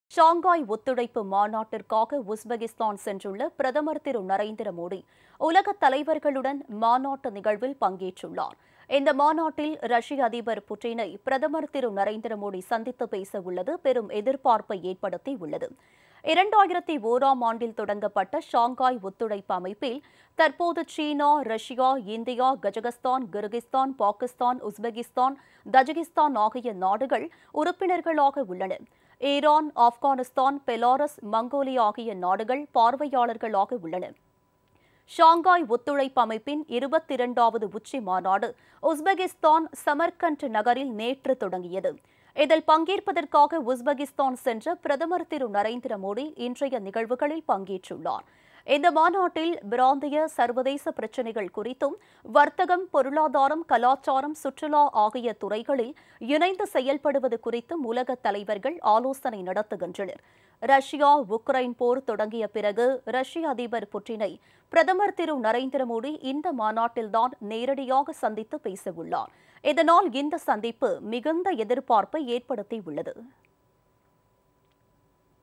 ரஷ்ய அதிபர் புதினை சந்திக்‍கிறார் பிரதமர் மோடி | PM Modi | SCO Summit 2022
SCOSummit2022 | PMModi | ரஷ்ய அதிபர் புதினை சந்திக்‍கிறார் பிரதமர் மோடி